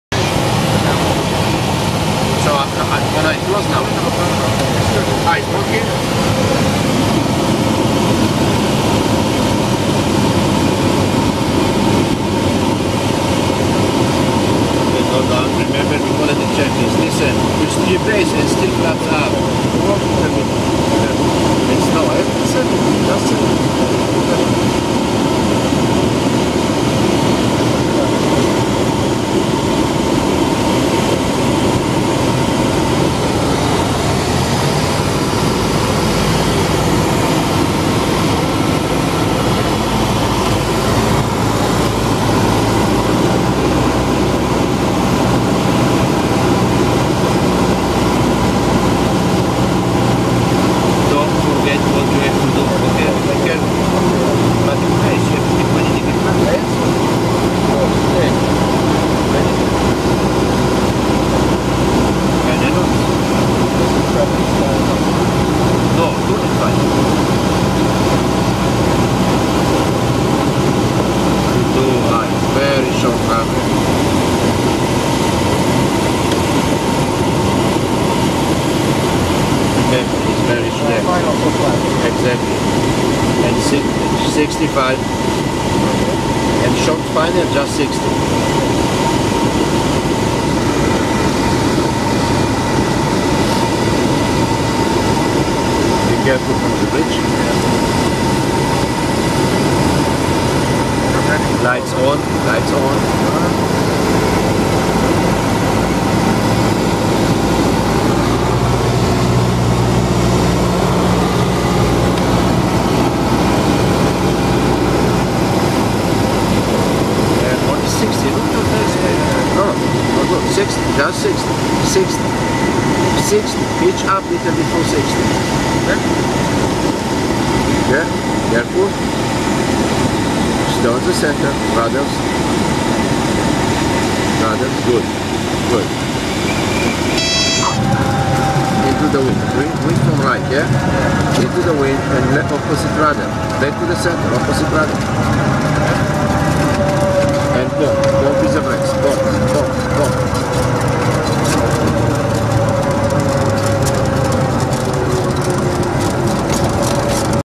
Goodspeed (42B) Short Field Landing
Short Field landing in Conneticut. I love the approach to rwy 14 here. Amazing views!